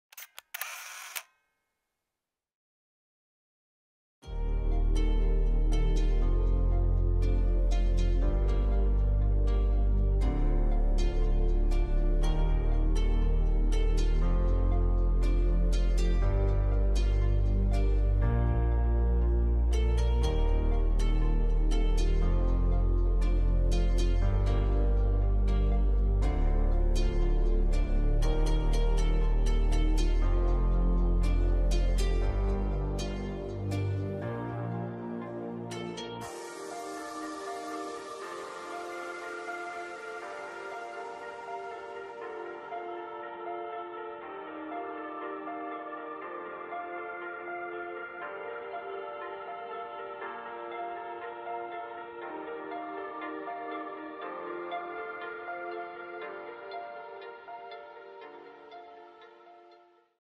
PHD Demo Reel 2016-17
This is a short reel of shots and artist i have worked with during my first year of shooting. Featuring major artist such as 21 Savage, Ralo, Young M.A., Lil Daddy, Shah, and Dae Dae. Also featuring shots from ASU new stadium, and football player workout and Dblock the Great.